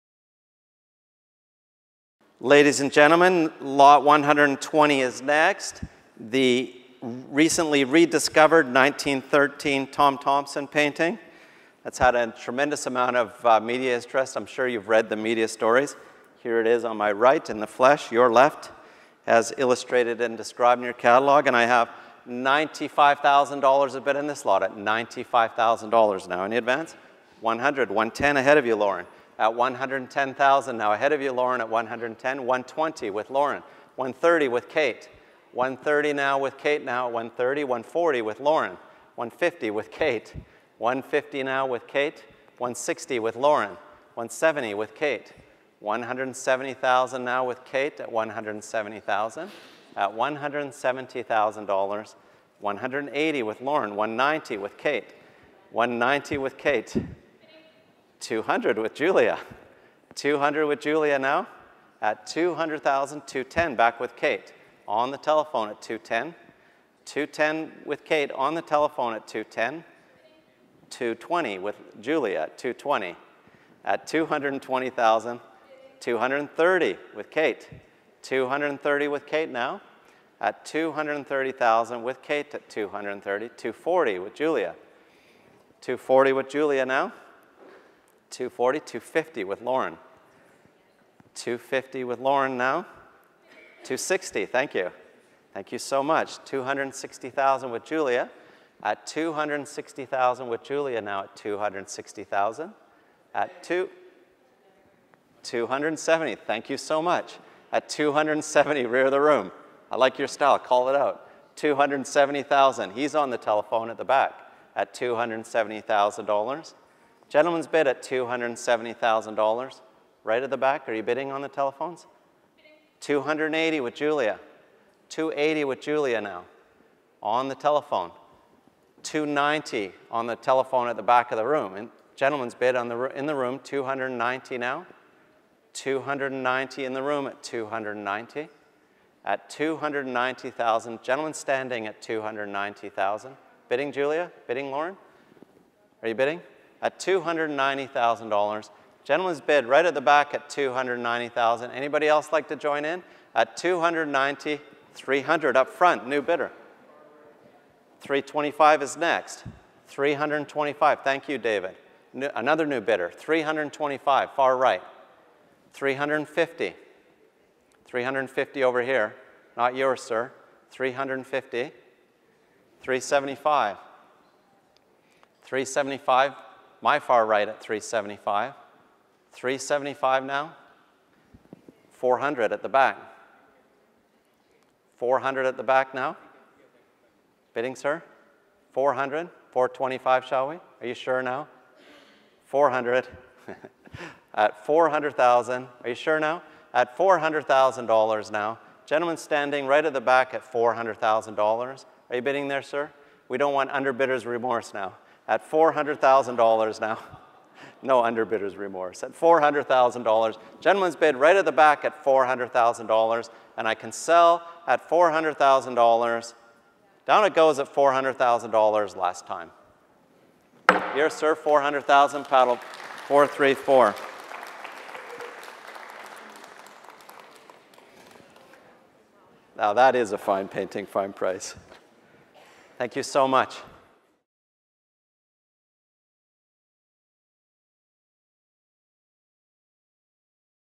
May 30, 2018, Tom Thomson, Sketch for Lake in Algonquin Park
With one of the most exciting backstories in Heffel auction history, Tom Thomson’s Sketch for Lake in Algonquin Park did not disappoint at our May 30, 2018 live auction. The painting was consigned from a woman in Edmonton who, for all her life, never believed her father’s Tom Thomson was real. Months ago, she gave it as a gag gift to a friend, who then brought it in to Heffel for appraisal. She was pleasantly surprised when its authenticity was confirmed, and the rest is history. Participation from a number of bidders on the phones and in the Toronto auction room propelled the final sale price for the important sketch to $481,250, more than three times its presale estimate. The two friends will be taking a Mediterranean cruise with the profits.

“There is always bit of magic on the auction floor, and tonight it was brought by the Tom Thomson that until recently was tucked away in a basement,” said David Heffel, President of Heffel Fine Art Auction House. “We believe that the results generated and this uplifting story will remind enthusiasts and collectors that there are still outstanding works of art to be discovered and enjoyed.”